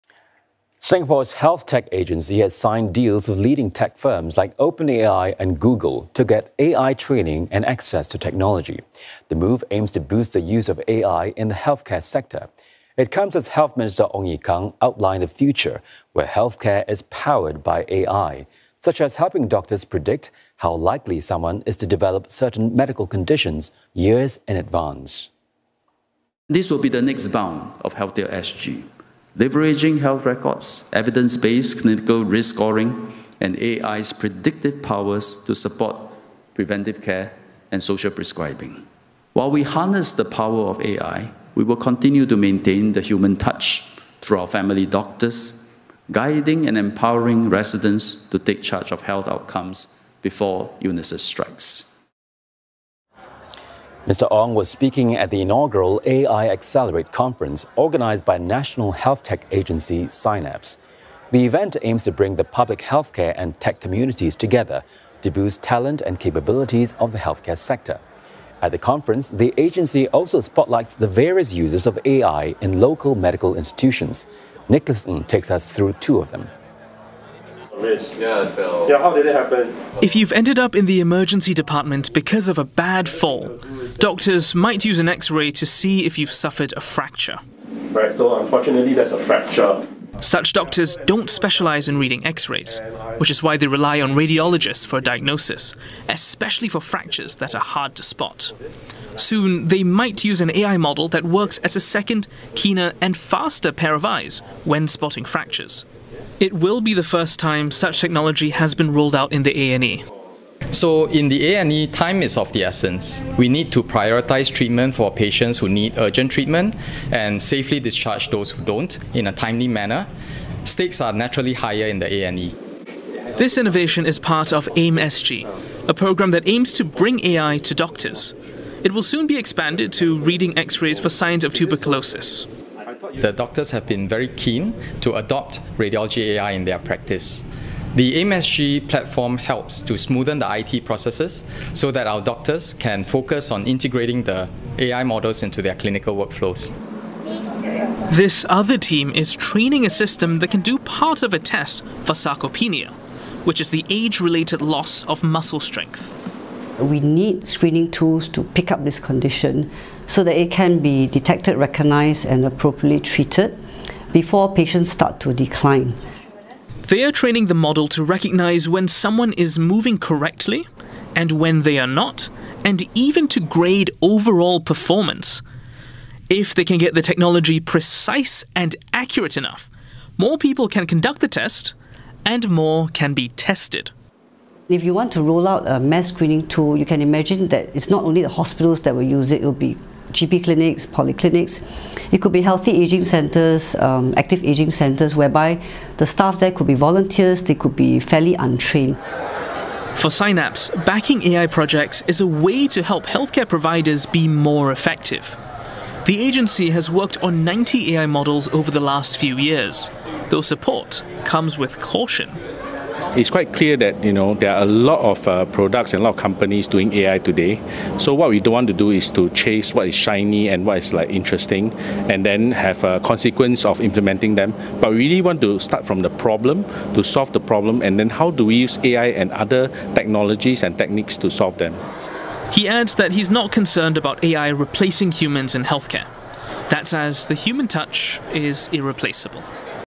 Singapore to work with leading tech firms to boost AI adoption in healthcare sector
Singapore's health tech agency Synapxe has signed deals with leading tech firms, such as OpenAI and Google, to get AI training and access to technology. The move aims to boost the use of AI in the healthcare sector. Health Minister Ong Ye Kung has outlined a future where healthcare is powered by AI, such as helping doctors predict how likely someone is to develop certain medical conditions. He was speaking at the inaugural AI Accelerate conference organised by Synapxe. Nicolas Ng reports.